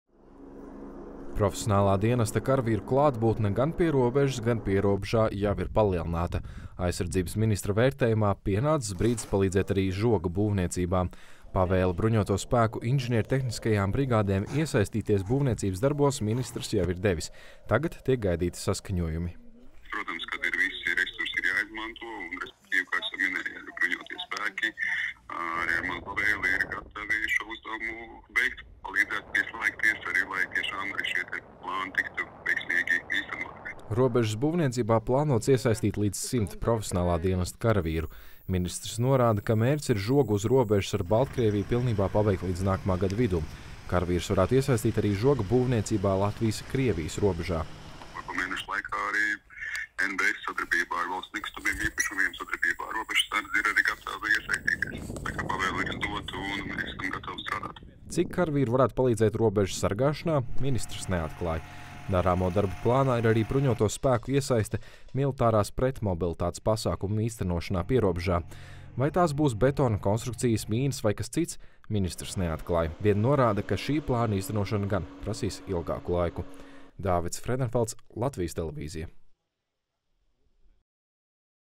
NBS karavīri palīdzēs žoga būvniecībā
Nacionālo bruņoto spēku profesionālā dienesta karavīri gatavi iesaistīties žoga būvniecībā uz mūsu valsts robežas, Latvijas Televīzijai atklāja aizsardzības ministrs Andris Sprūds. Arvien vairāk karavīru plānots iesaistīt arī robežas apsargāšanā.